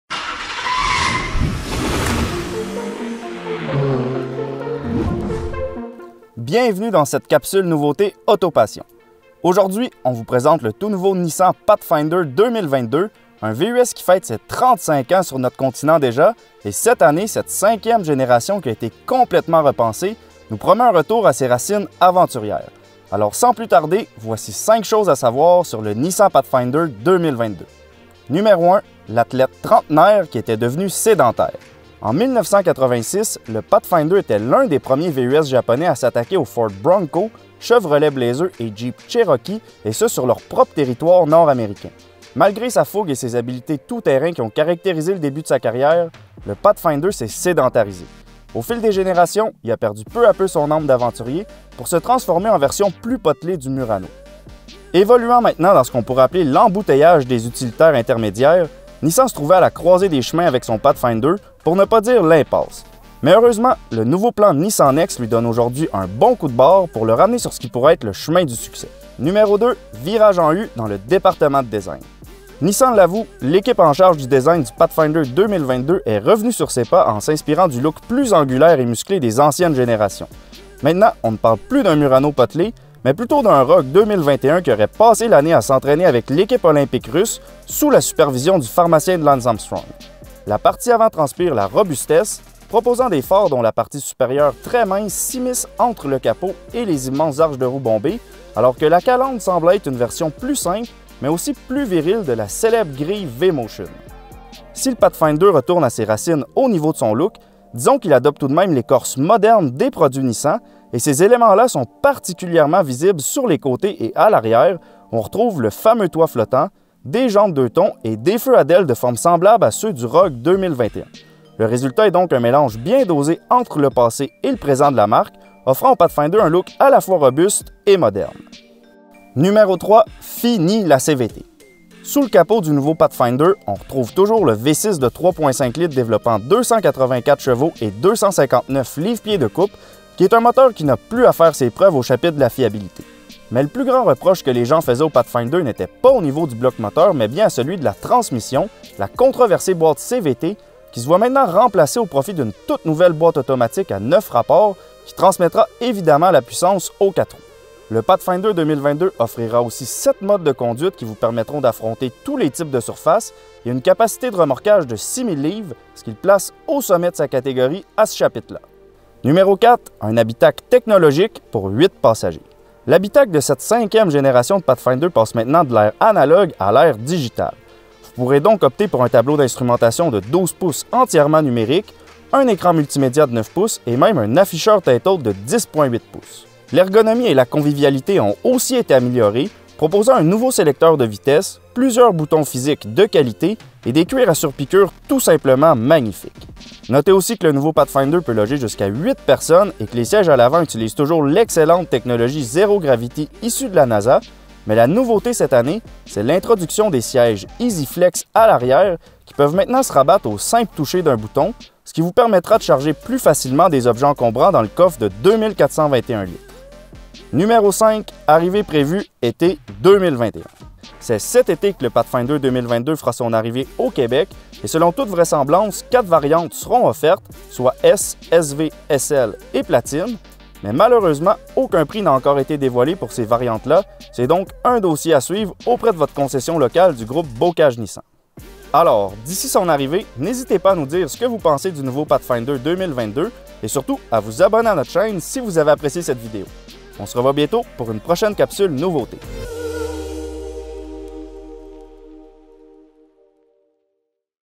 5 Choses À Savoir – Nissan Pathfinder 2022
Aujourd’hui, on vous présente 5 choses à savoir sur le Nissan Pathfinder 2022, un VUS qui fête ses 35 ans sur notre continent déjà, et cette année, cette 5e génération qui a été complètement repensée nous promet un retour à ses racines aventurières! Comment le trouvez-vous?